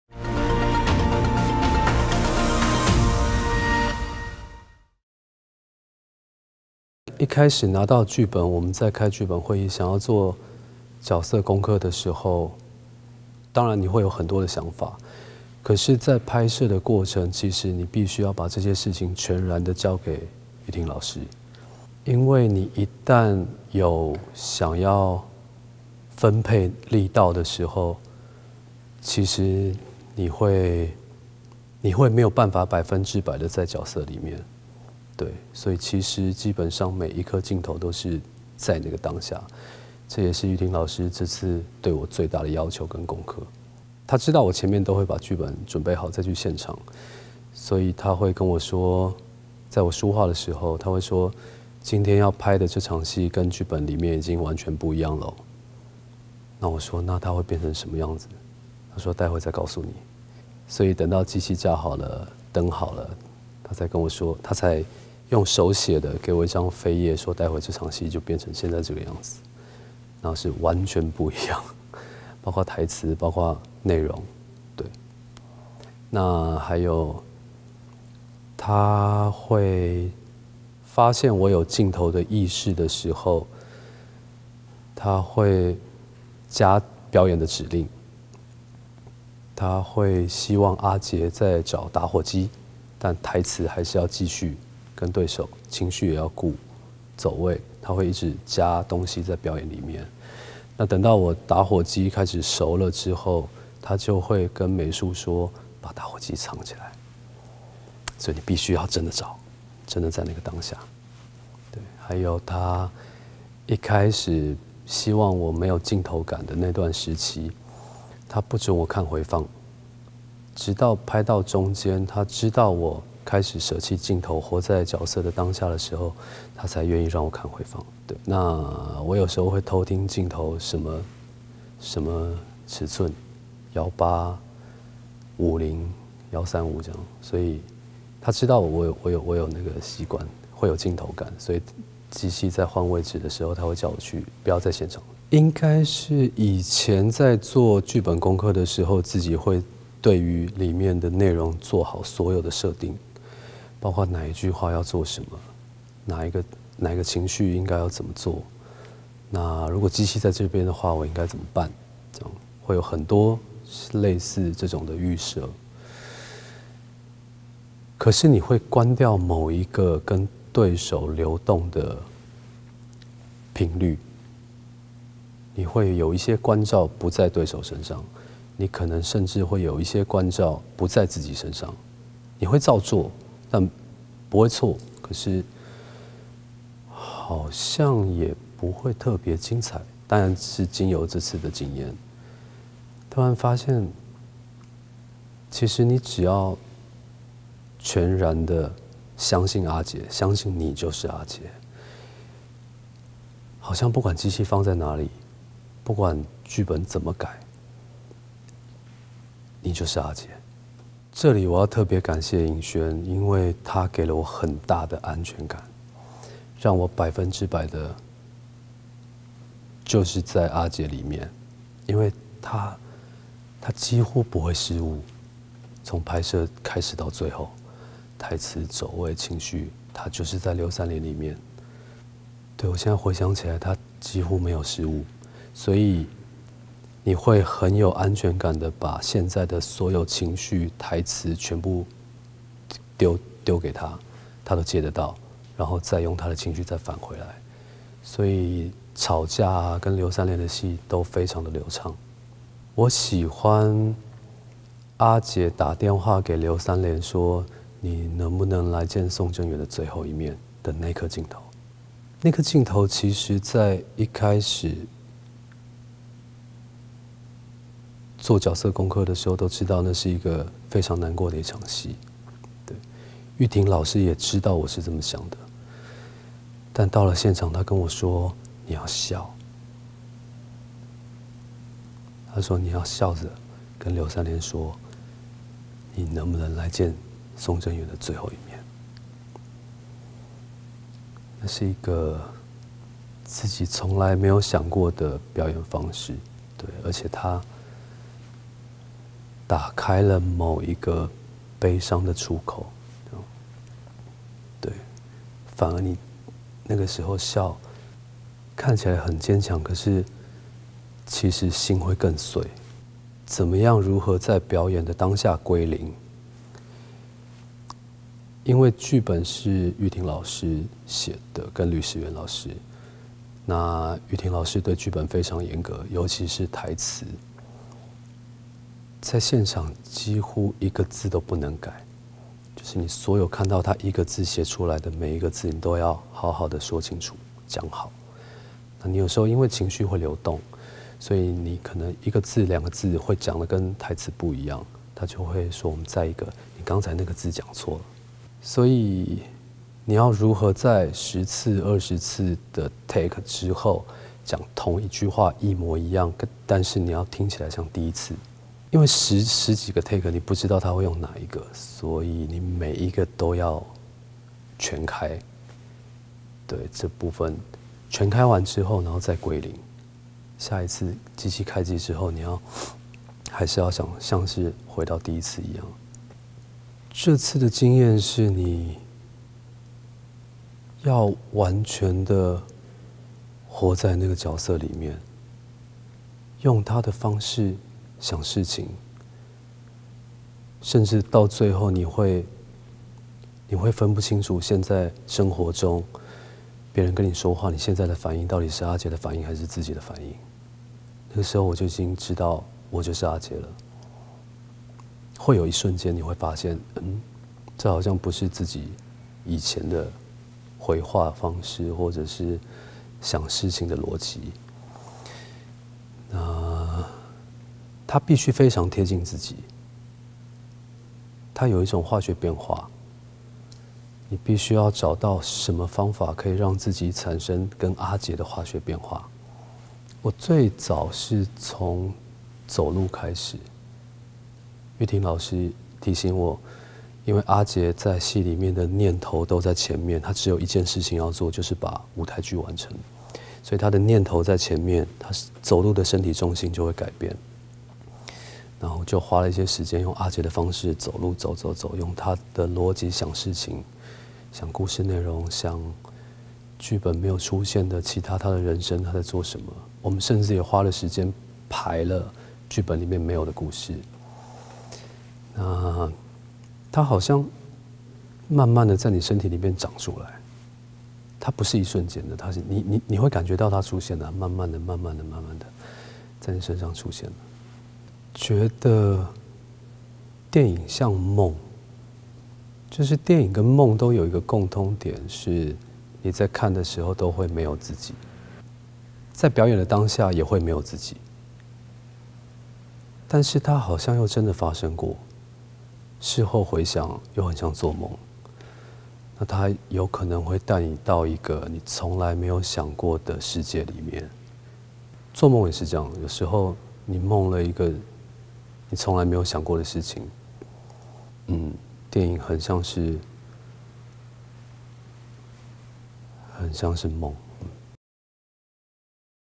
金馬55榮耀時刻︱最佳男主角入圍訪談影片：邱澤
電影像夢，電影和夢都有一個共通點是
　你在看的時候都會沒有自己，在表演的當下也會沒有自己，
　但是它好像又真的發生過，
　它可能會帶你到一個從來沒有想過的世界裡面。」